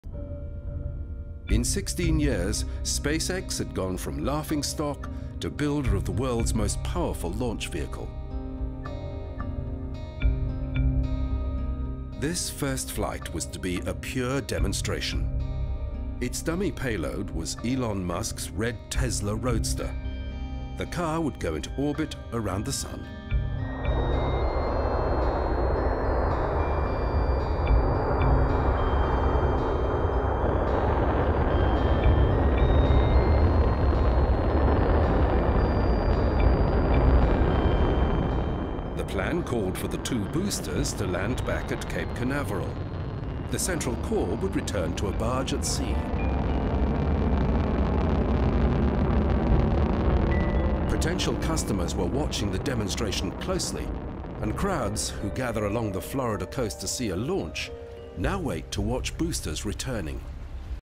Falcon Heavy: SpaceX's Epic First Flight
Experience the incredible journey of SpaceX's Falcon Heavy rocket, from its development to its groundbreaking first flight carrying a Tesla Roadster into space! Witness the engineering marvel and the excitement of this ambitious project.